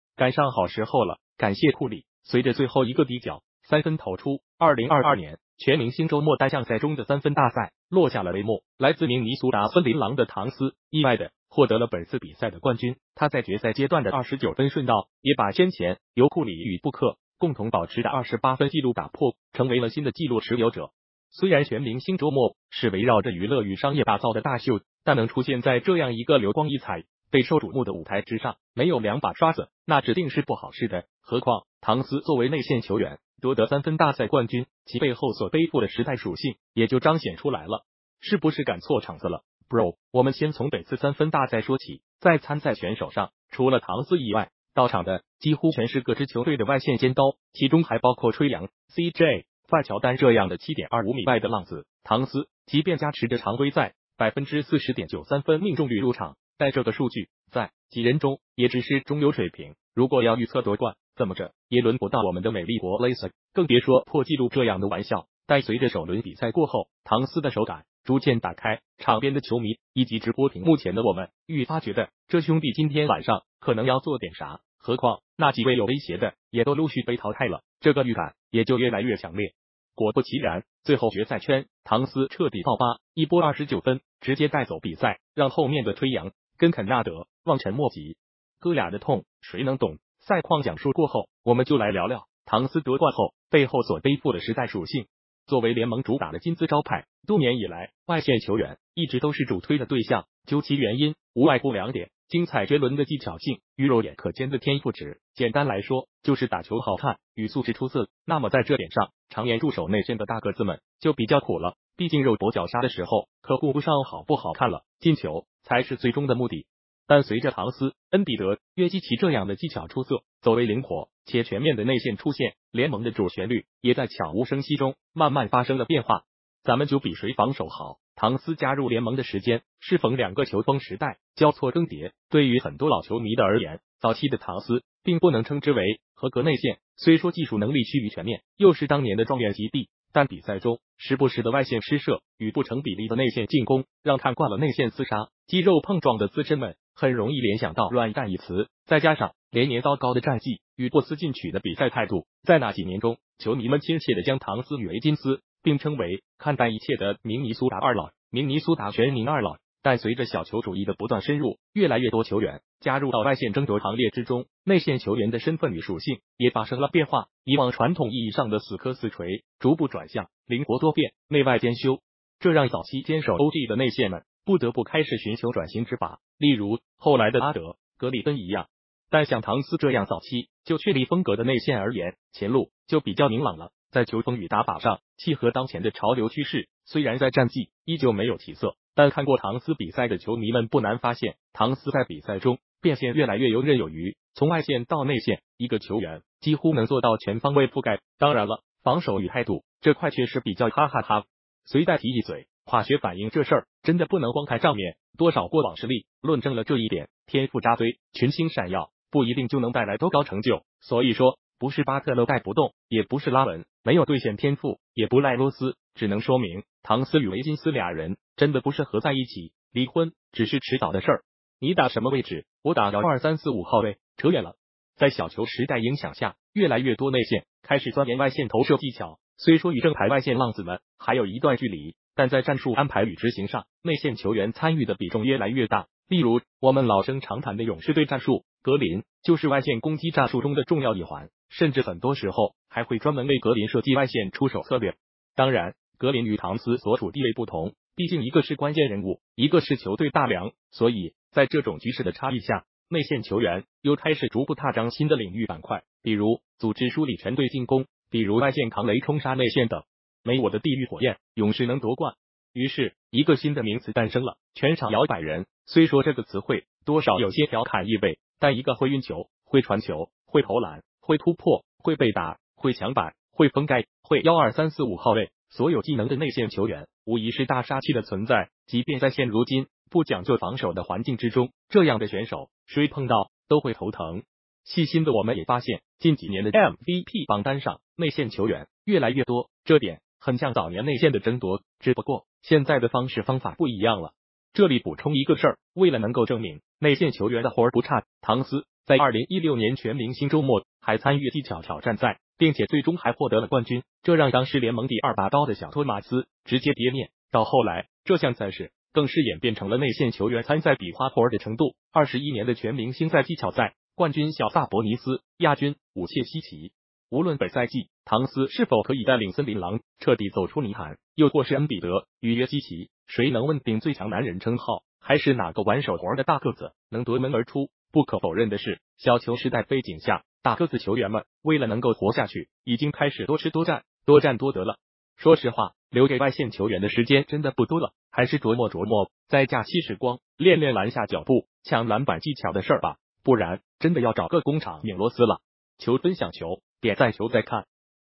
留给后卫们的时间真的不多了
更多视屏可在网站搜索： “赶上好时候了，感谢库里”随着最后一个底角三分投出，2022年全明星周末单项赛中的三分大赛落下了帷幕，来自明尼苏达森林狼的唐斯“意外”的获得了本次比赛的冠军，他在决赛阶段的29分顺道也把先前由库里与布克共同保持的28分记录打破，成...